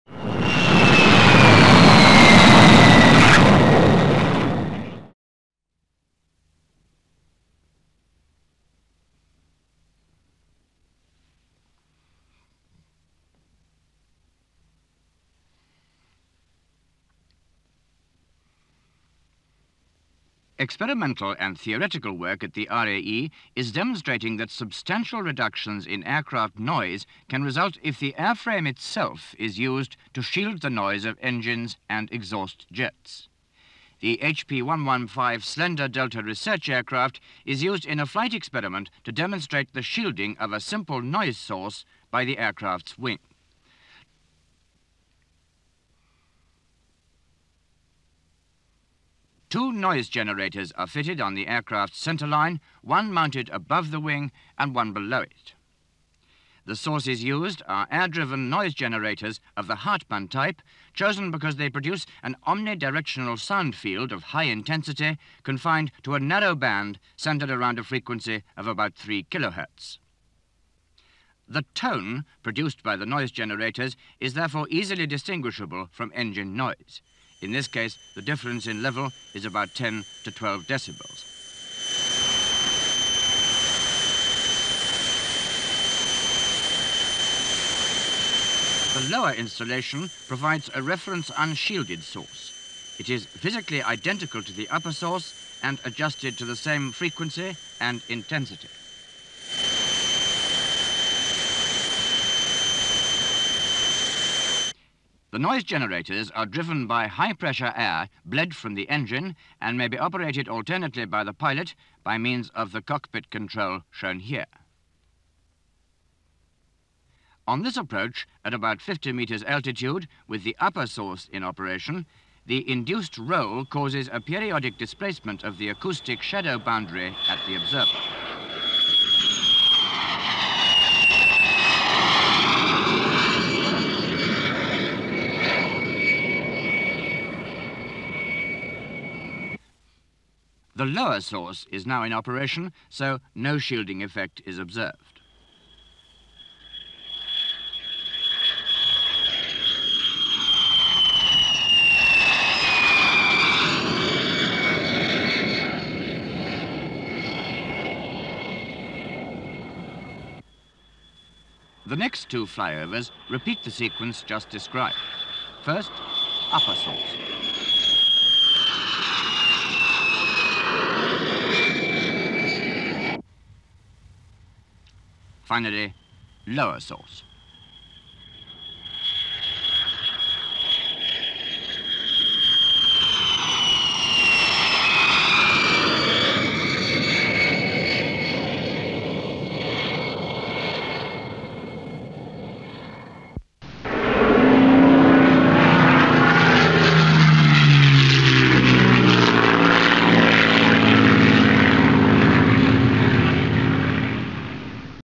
HP115 Trials from The Royal Aircraft Establishment
Trials carried out by the Royal Aircraft Establishment into reduction of noise from aircraft.